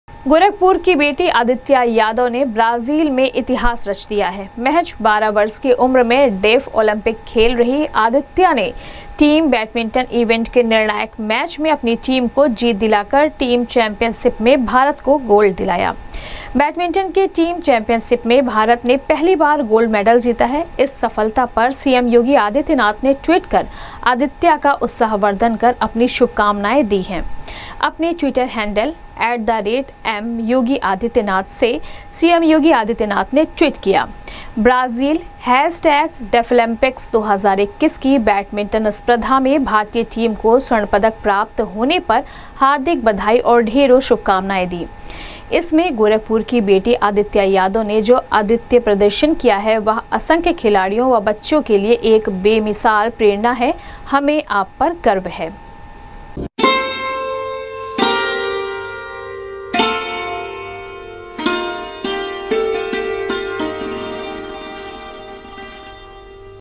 डेफ ओलंपिक की गोल्ड मेडलिस्ट गोरखपुर की बेटी आदित्या के अद्वितीय प्रदर्शन पर सीएम योगी ने आदित्या
डेफ ओलंपिक की गोल्ड मेडलिस्ट गोरखपुर की बेटी आदित्या के अद्वितीय प्रदर्शन पर सीएम योगी ने आदित्या का बढ़ाया उत्साह, आदित्या के लिए सीएम योगी ने किया ट्वीट, बोले हमें गर्व है आप पर